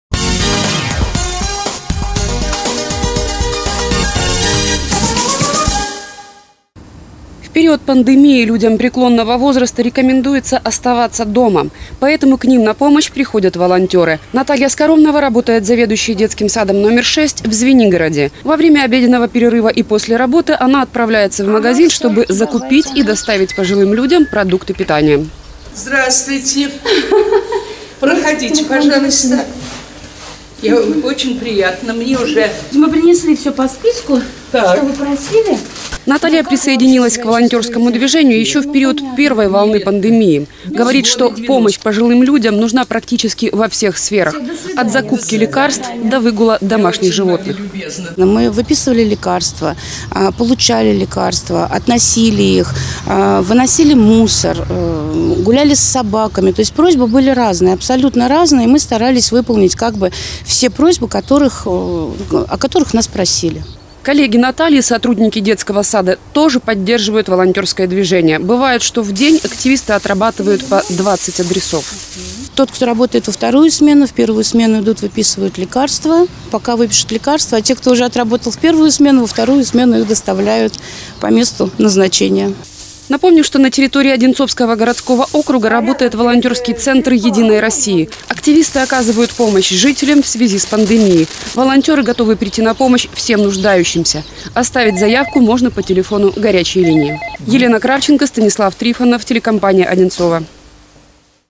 Помощь нуждающимся
В Одинцовском городском округе продолжается активная волонтерская деятельность. Добровольцы помогают пожилым людям в период пандемии.